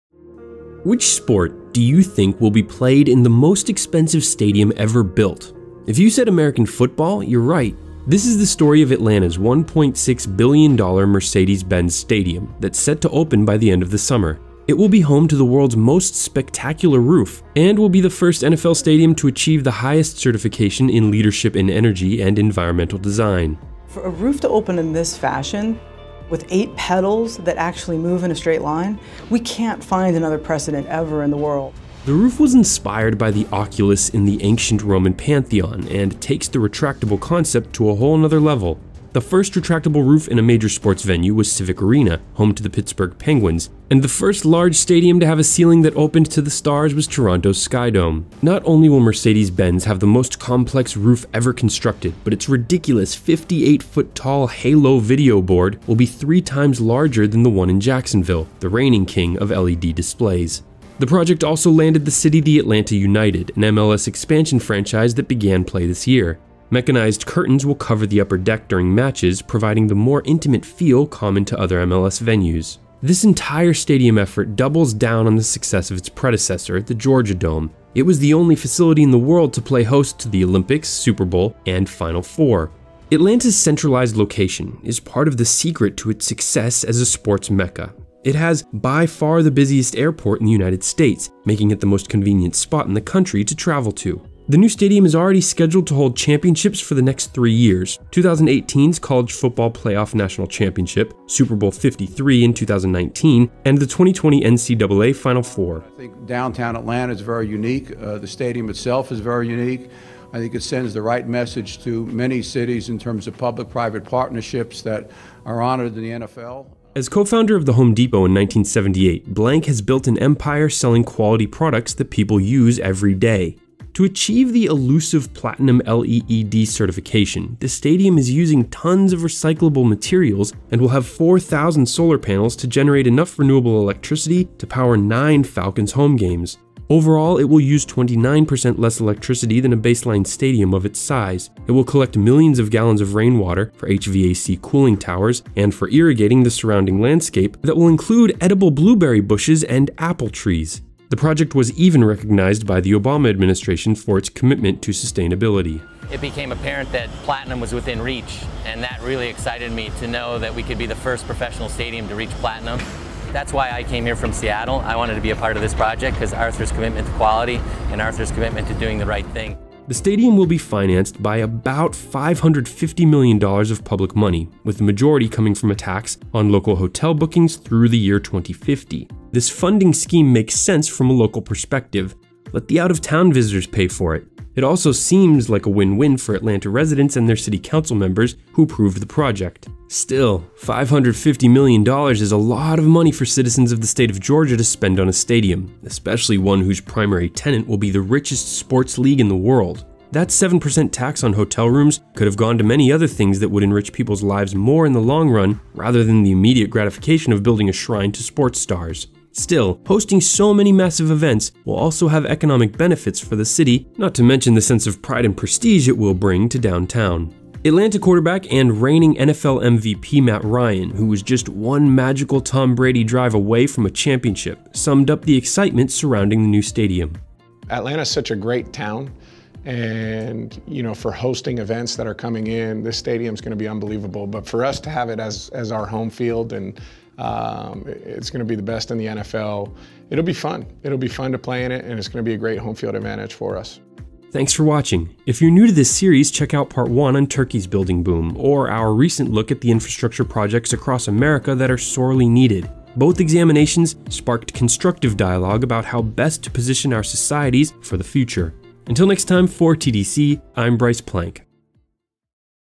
Super Bowl LIII [53] Stadium: MEGAPROJECTS
Atlanta's Mercedes-Benz Stadium is the most expensive stadium ever built. The $1.6 billion price tag was driven by a spectacular retractable roof design, an LED video board that is by far the world's largest, and the pursuit of LEED platinum certification.

Video by Bryce Plank and Robin West

Most videos courtesy of Mercedes Benz stadium/Atlanta Falcons

Script:
Which sport do you think will be played in the most expensive stadium ever built? If you said American football, you’re right.

This is the story of Atlanta’s $1.6 billion Mercedes Benz Stadium that’s set to open by the end of the summer. It will be home to the world’s most spectacular roof and will be the first NFL stadium to achieve the highest certification in Leadership in Energy and Environmental Design.

“For a roof to open in this fashion, with eight petals that actually move in a straight line, we can’t find another precedent ever in the world.”

The roof was inspired by the oculus in the ancient Roman Pantheon and takes the retractable concept to a whole other level. The first retractable roof in a major sports venue was Civic Arena — home to the Pittsburgh Penguins — and the first large stadium to have a ceiling that opened to the stars was Toronto’s SkyDome.

Not only will Mercedes Benz have the most complex roof ever constructed, but it’s ridiculous 58-foot-tall halo video board will be three times larger than the one in Jacksonville, the reigning king of LED displays.

The project also landed the city the Atlanta United, an MLS expansion franchise that began play this year. Mechanized curtains will cover the upper deck during matches, providing the more intimate feel common to other MLS venues.

This entire stadium effort doubles down on the success of its predecessor, The Georgia Dome. It was the only facility in the world to play host to the Olympics, Super Bowl, and Final Four.

Atlanta’s centralized location is part of the secret to its success as a sports mecca. It has by far the busiest airport in the United States, making it the most convenient spot in the country to travel to.

The new stadium is already scheduled to hold championships for the next three years: 2018’s College Football Playoff National Championship, Super Bowl LIII in 2019, and the 2020 NCAA Final Four.

[Falcons’ Owner Arthur Blank] “I think downtown Atlanta is very unique, I think the stadium itself is very unique. I think it sends the right message to many cities in terms of public-private partnerships that are honored in the NFL.

As co-founder of The Home Depot in 1978, Blank has built an empire selling quality products that people use everyday.

To achieve the elusive platinum LEED certification, the stadium is using tons of recyclable materials and will have 4,000 solar panels to generate enough renewable electricity to power nine Falcons home games. Overall it will use 29% less electricity than a baseline stadium of its size. It will collect millions of gallons of rainwater for HVAC cooling towers and for irrigating the surrounding landscape that will include edible blueberry bushes and apple trees. The project was even recognized by the Obama administration for its commitment to sustainability.

[Stadium General Manager Scott Jenkins] “It became apparent that platinum was within reach and that really excited me to know that we could be the first professional stadium to reach platinum. That’s why I came here from Seattle, I wanted to be a part of this project, because of Arthur’s commitment to quality and Arthur’s commitment to doing the right thing.”

The stadium will be financed by about $550 million of public money, with the majority coming from a tax on local hotel bookings through the year 2050.

This funding scheme makes sense from a local perspective, let the out of town visitors pay for it. It also seems like a win-win for Atlanta residents and their city council members who approved the project.

Still, $550 million is a lot of money for citizens of the state of Georgia to spend on a stadium, especially one whose primary tenant will be the richest sports league in the world. That 7% tax on hotel rooms could’ve gone to many other things that would enrich people’s lives more in the long run rather than the immediate gratification of building a shrine to sports stars. Still, hosting so many massive events will also have economic benefits for the city, not to mention the pride and prestige it will add to downtown.

Atlanta quarterback and reigning NFL MVP Matt Ryan — who was just one magical Tom Brady drive away from a championship — summed up the excitement surrounding the new stadium:
[Matt Ryan] “Atlanta’s such a great town..."